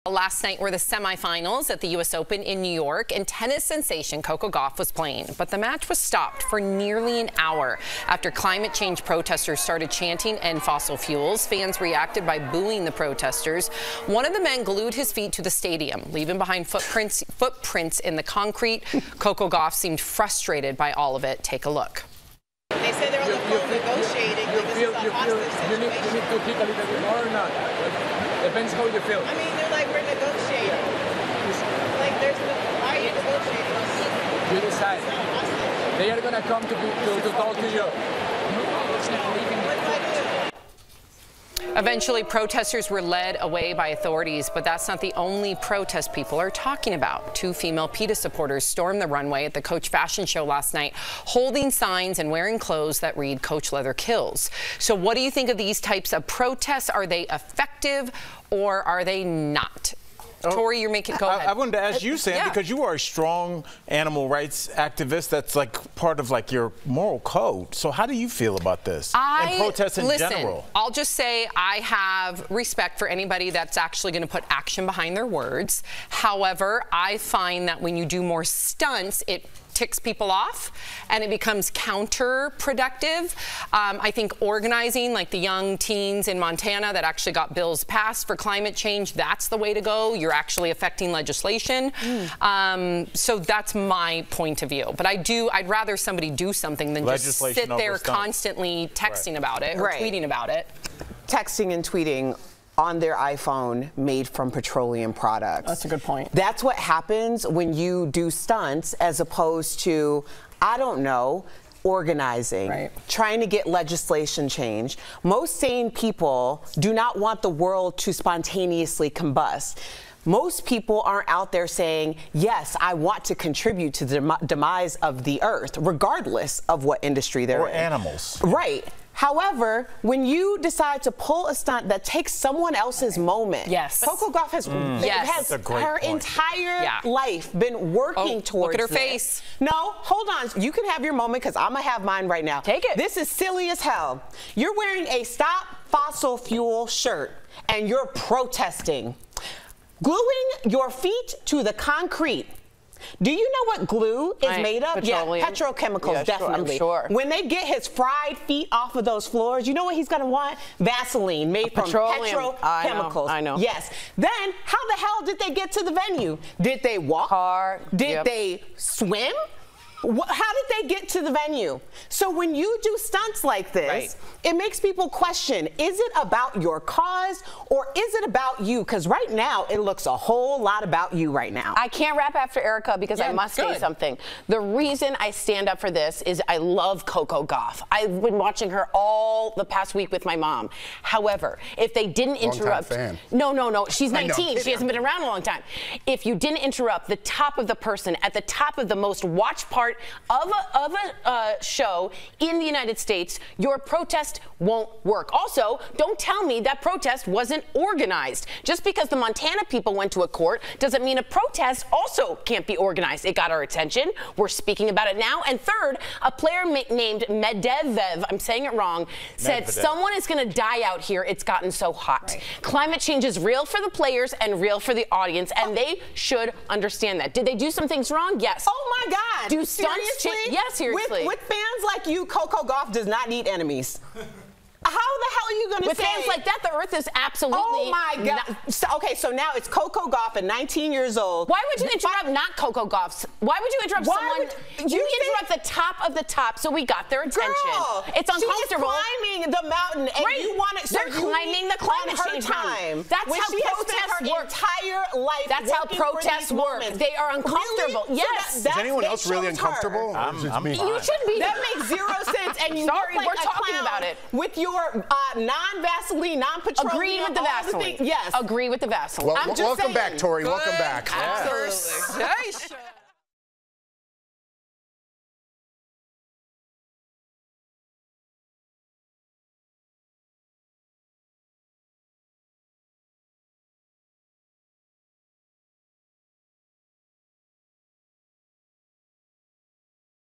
Climate Change Protesters Interrupt Coco Gauff During U.S. Open in New York
The semi-finals of the U.S. Open in New York happened on Thursday night and tennis sensation Coco Gauff was playing. The match was stopped for nearly an hour after Climate Change protesters started chanting ‘End Fossil Fuels’ and one of them glued their feet to the stadium.

DBL’s Erica Cobb and Tory Shulman have differing opinions on the incident. What do you think?

Make DBL Your Daytime Destination | Weekdays!
 12:30 p.m. - 1 p.m. ET
   2:00 p.m. - 6 p.m. ET
   7:30 p.m. - 8 p.m. ET

🔴 DailyBlastLive.com